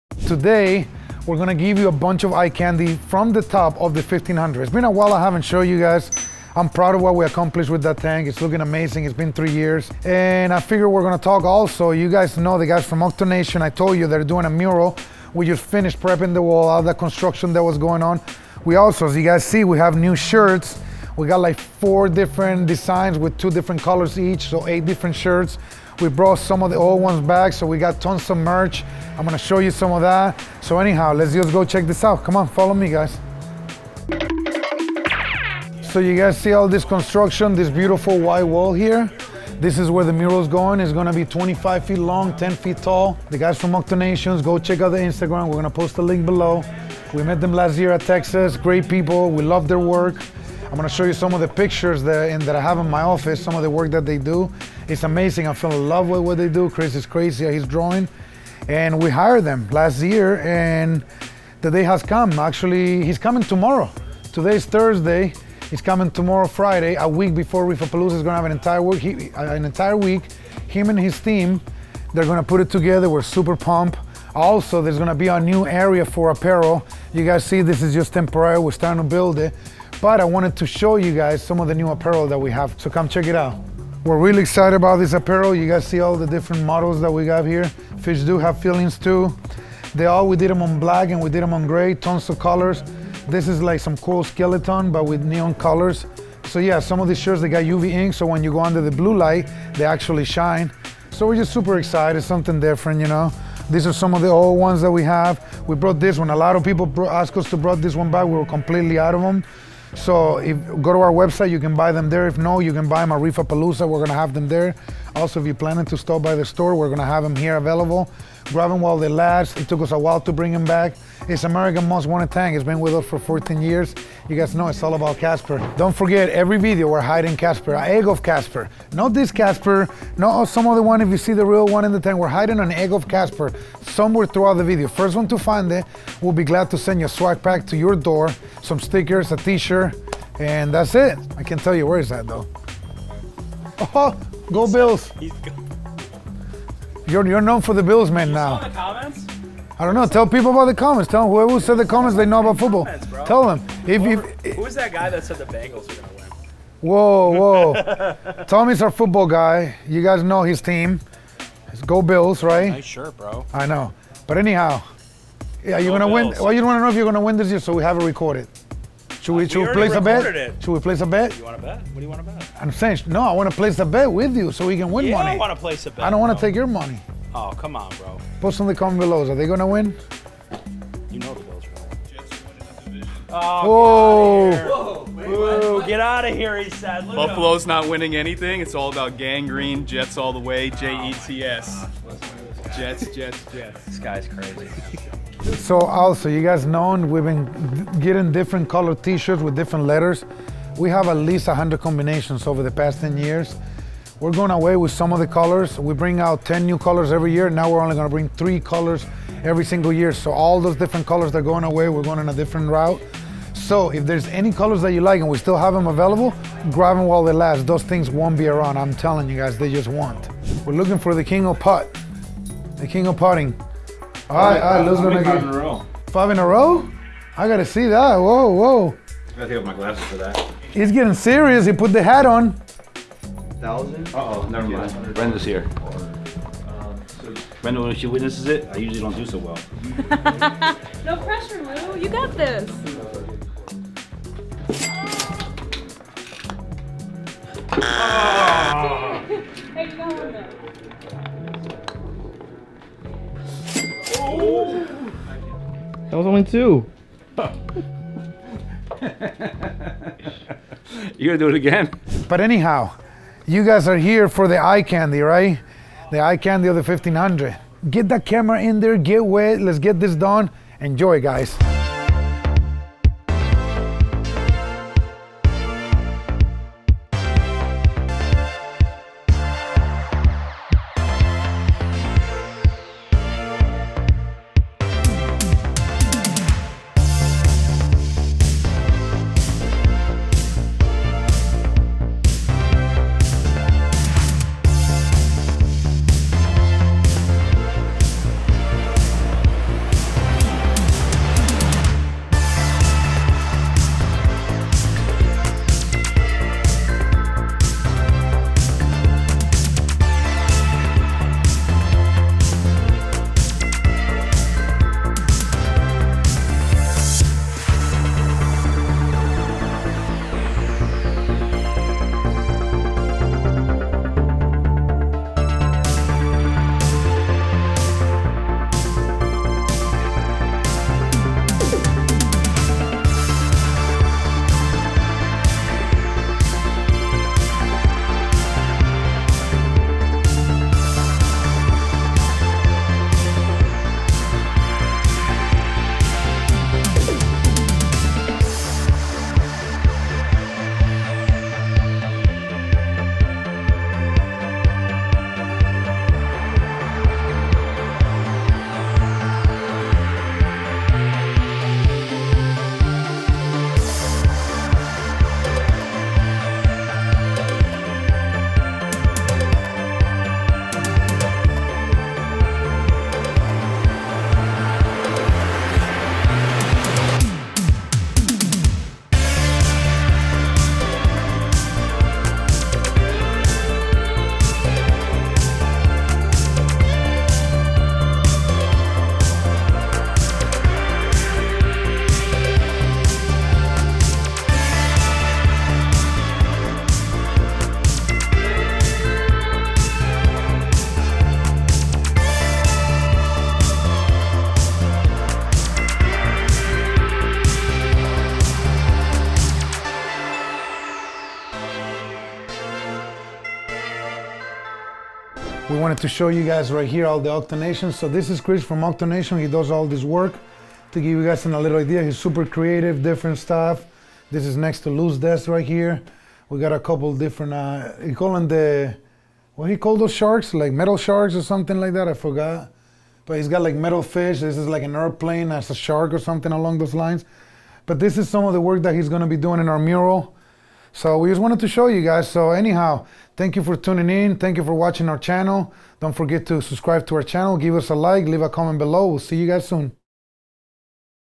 1500 Gallons of INSANE Coral Reef!
How about some amazing eye candy of our 1500 gallon display reef aquarium from the top? Before we get to that, Victor gives us an update on the Octonation wall and what else is happening at WWC.